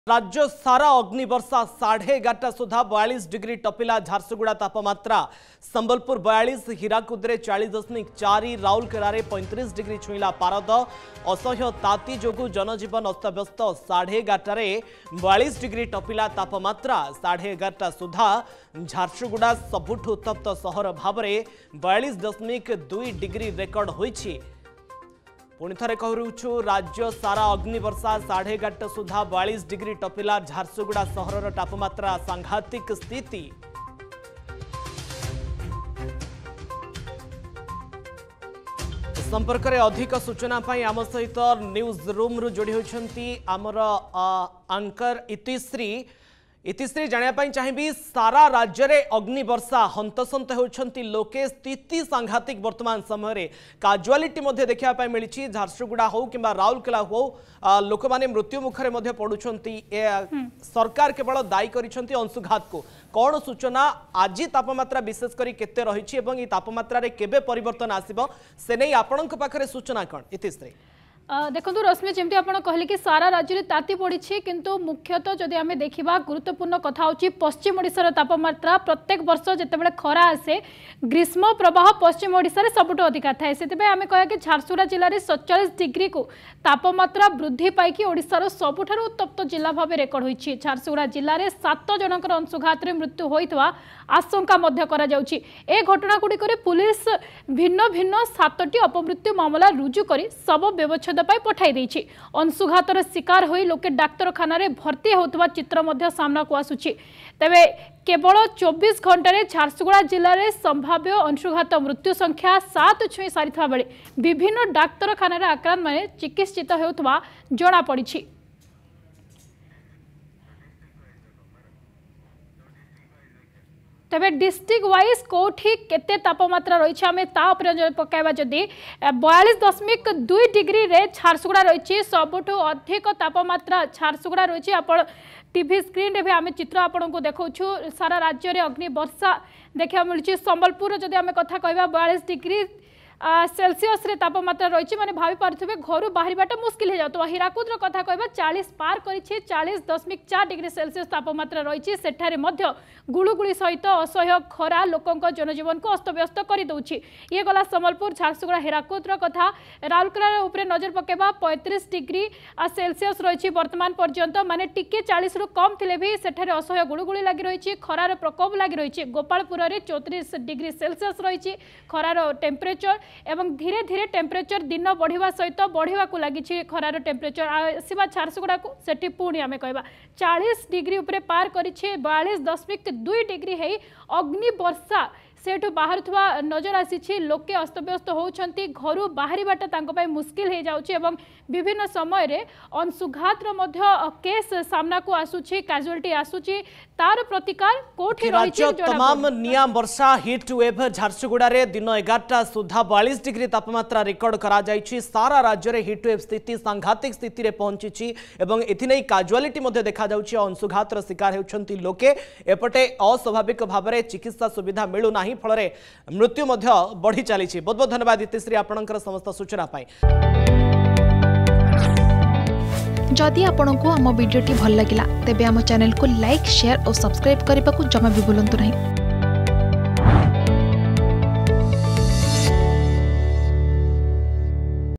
ରାଜ୍ୟ ସାରା ଅଗ୍ନିବର୍ଷା || Severe Heatwave in Odisha
ରାଜ୍ୟ ସାରା ଅଗ୍ନିବର୍ଷା ।  `ପଶ୍ଚିମ ଓଡ଼ିଶାରେ ନିୟମିତ ବୃଦ୍ଧି ପାଉଛି ତାପମାତ୍ରା' ।